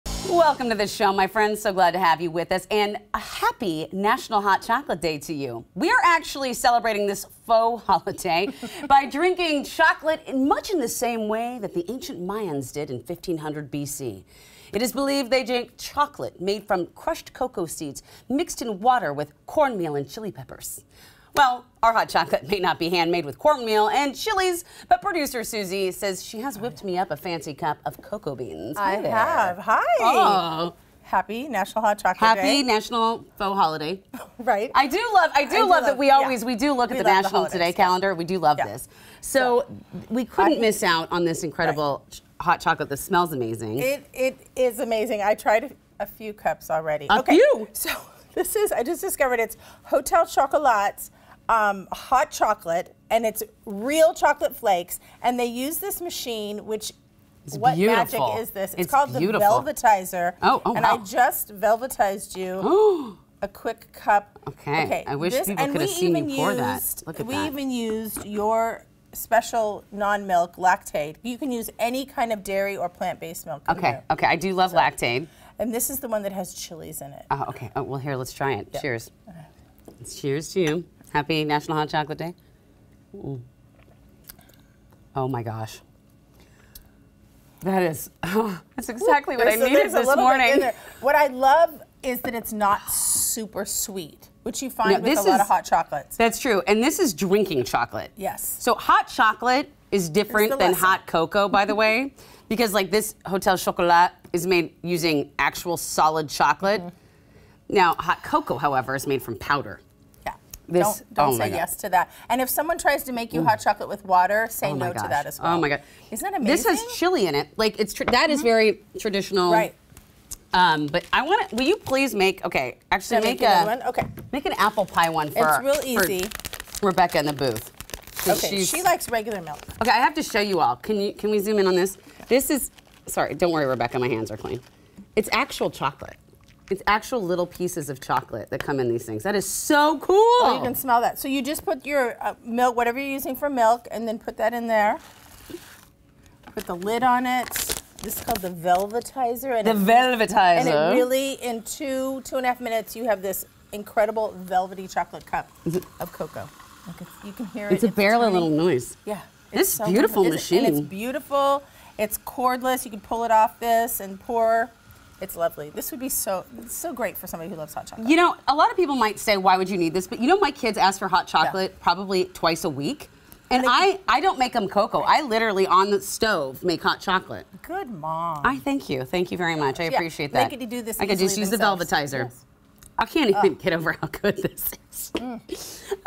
It’s National Hot Chocolate Day and here’s how to make it velvety smooth - New Day NW
Hotel Chocalat’s Velvetiser plus incredible chocolate flakes make a perfect cup every time.